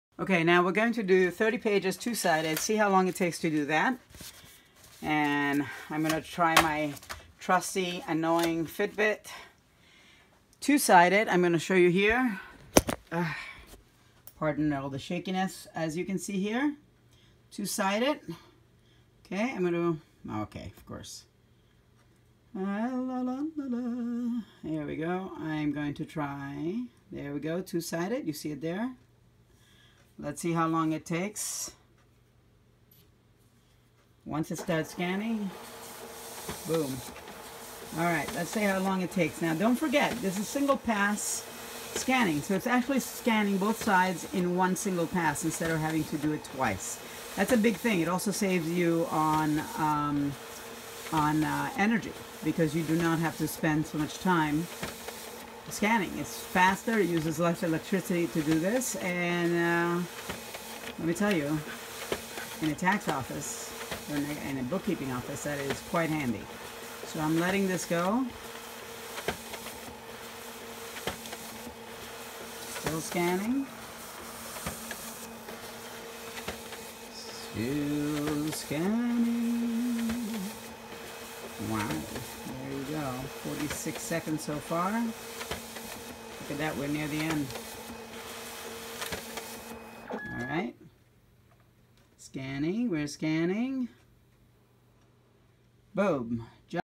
HP ScanJet Pro 4500 Fn1 Evaluation - 6 of 7
Checking out the HP ScanJet Pro 4500 Fn1. Excited to see how it fits into our workflow and how it will help us.